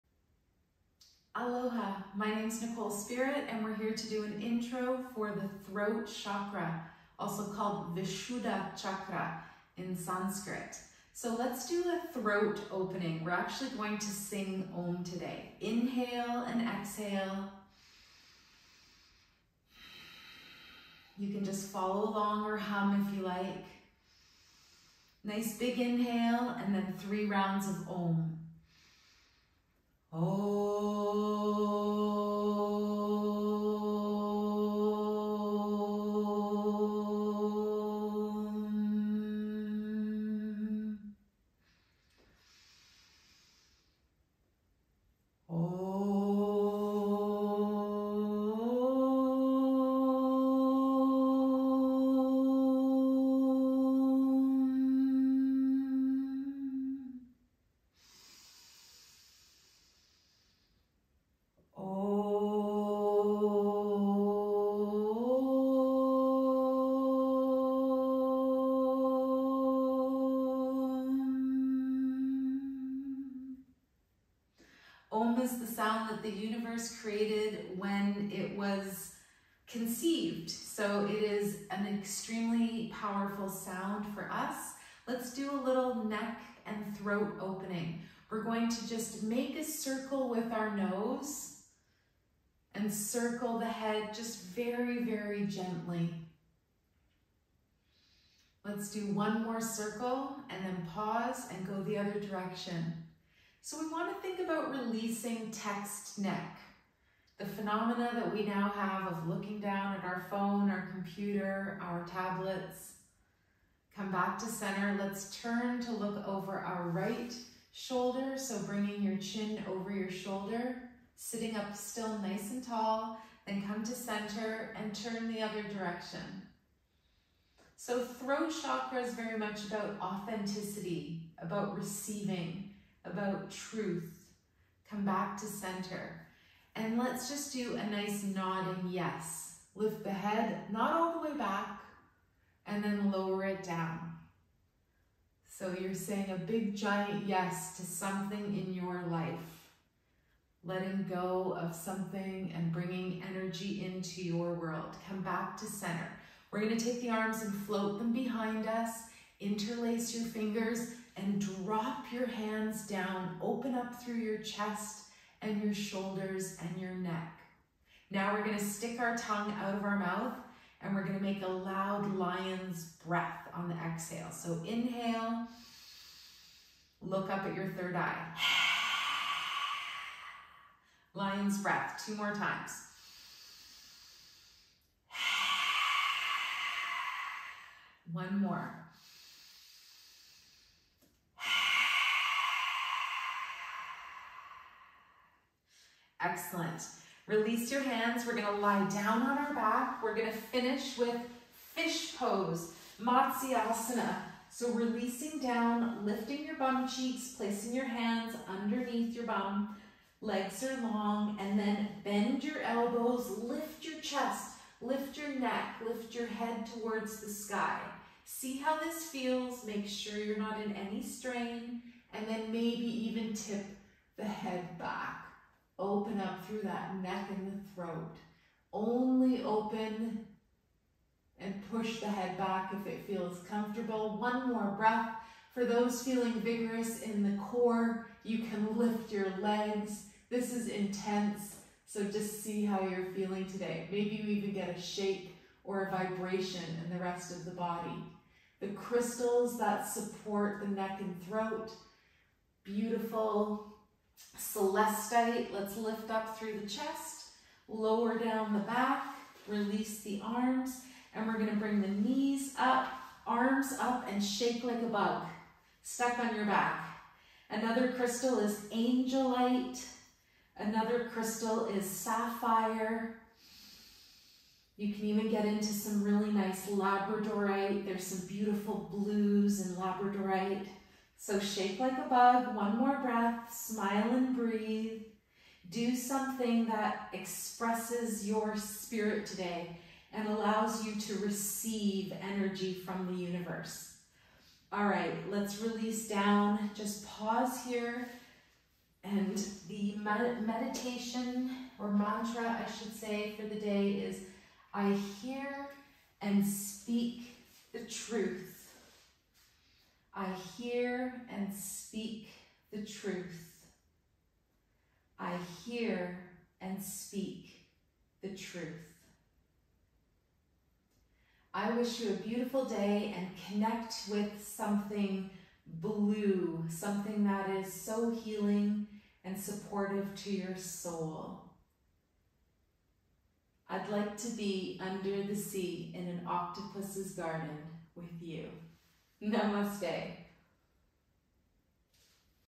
Throat Charka | 5 min
Enjoy Day 5 of 7 with this short and sweet video about the Throat Chakra with Nicole Spirit. Blue is the vibration of the neck and throat and relates to your expression and spirit. Put on something Blue and Pitter-Patter, Let's Get Atter.💙🩵💙